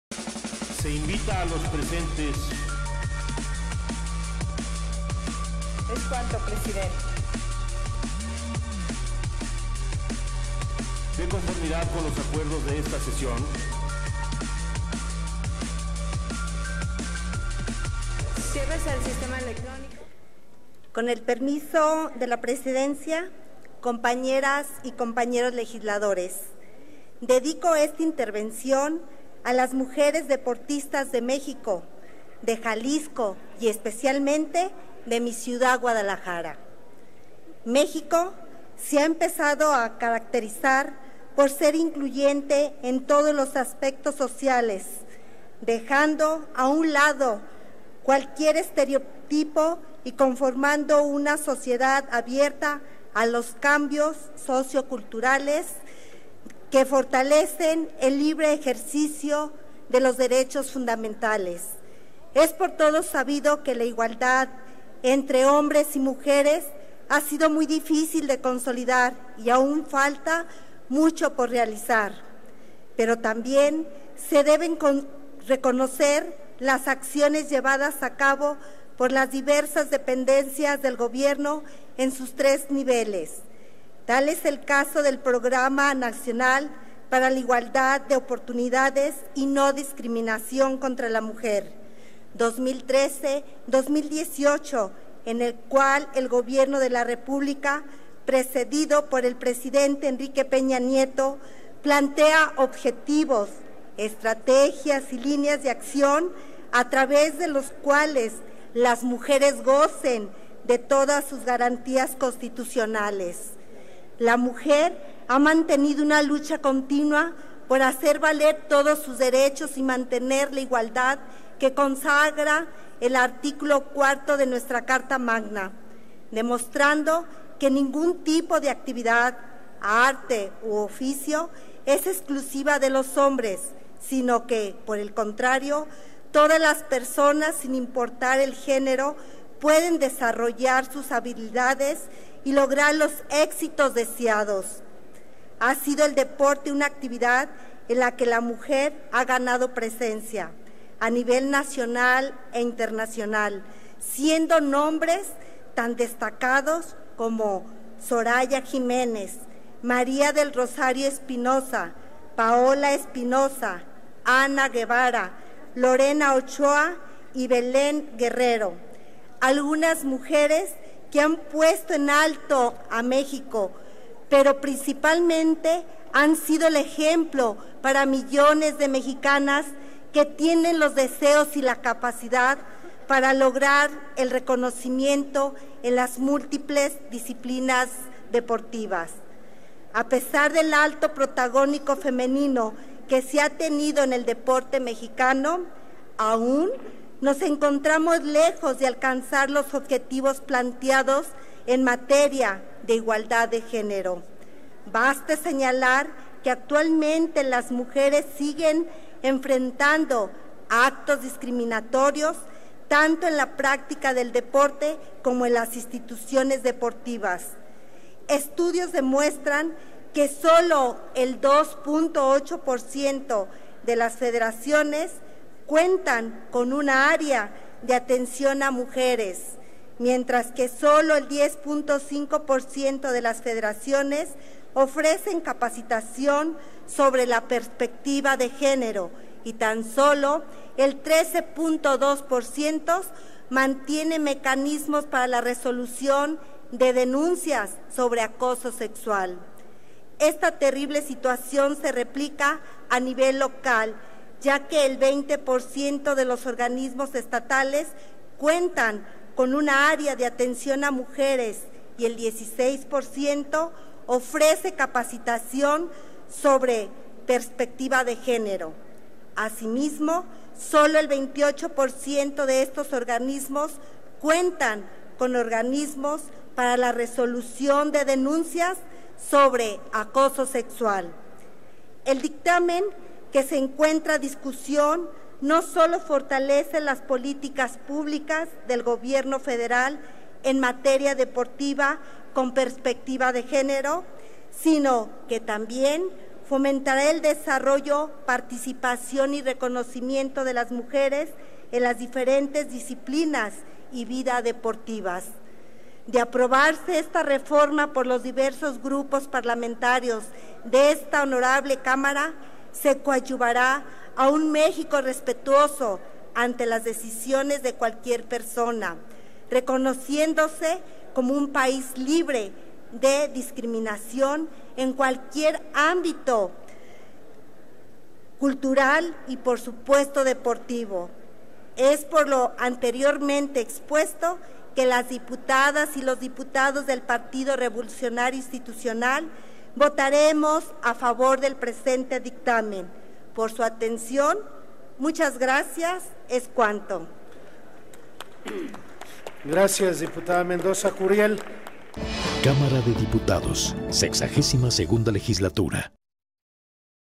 Dip. Leticia Mendoza (PRI) - Ley General para la Igualdad entre Mujeres y Hombres
La Dip. María Leticia Mendoza Curiel (PRI) destaca la participación de las mujeres deportistas y se pronuncia por el libre ejercicio de los derechos fundamentales. Igualmente, recuerda los objetivos del programa nacional para la igualdad de las mujeres y la lucha emprendida por dicho sector para hacer valer su integridad y derechos. Recuerda a las mujeres deportistas mexicanas que han puesto en alto el nombre del país en las competencias deportivas y advierte sobre los rezagos existentes. Igualmente, refiere que solo el 2.8% de las federaciones cuentan con un área de atención hacia las mujeres y asegura que dicha situación se replica a nivel local. Finalmente, se pronuncia por un país libre de cualquier tipo de discriminación.